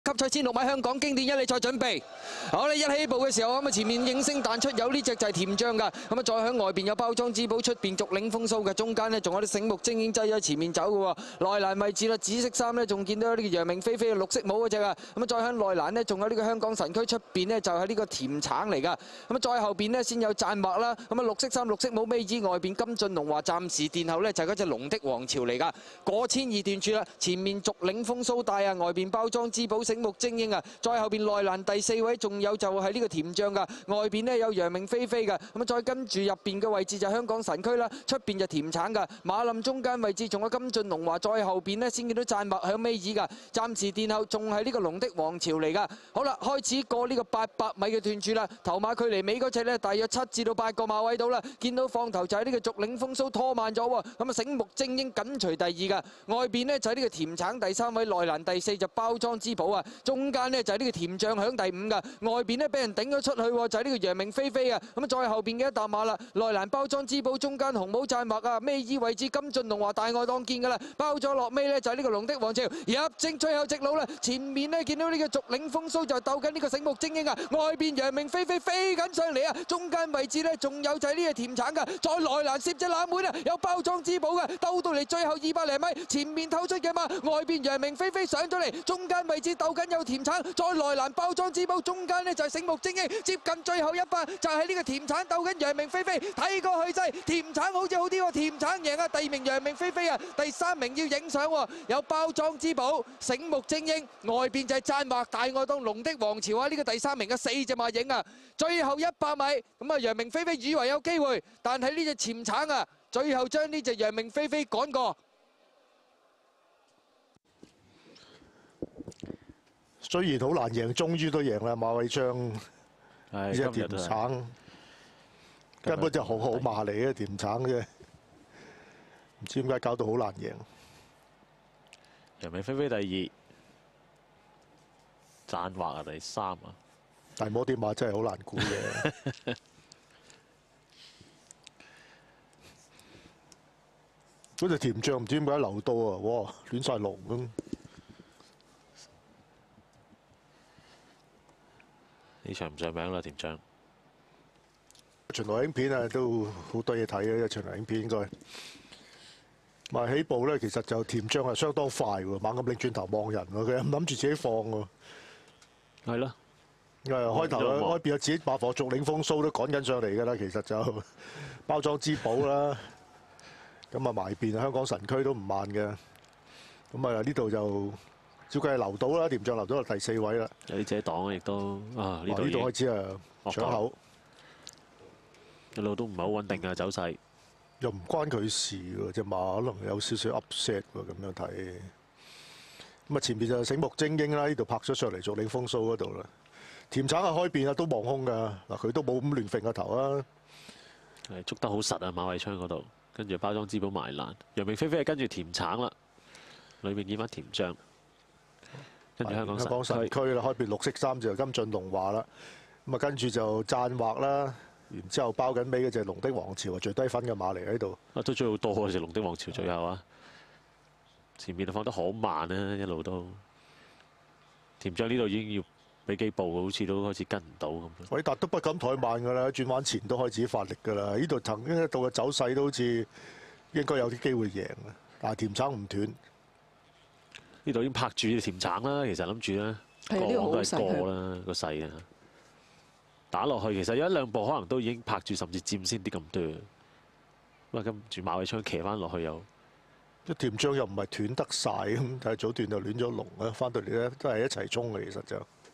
[2012 Mercedes-Benz 香港經典一哩賽]「甜橙」勝出Mercedes-Benz 香港經典一哩賽 2012
霍利時馬房的「甜橙」在騎師馬偉昌胯下勝出Mercedes-Benz香港經典一哩賽（香港一級賽1600米）。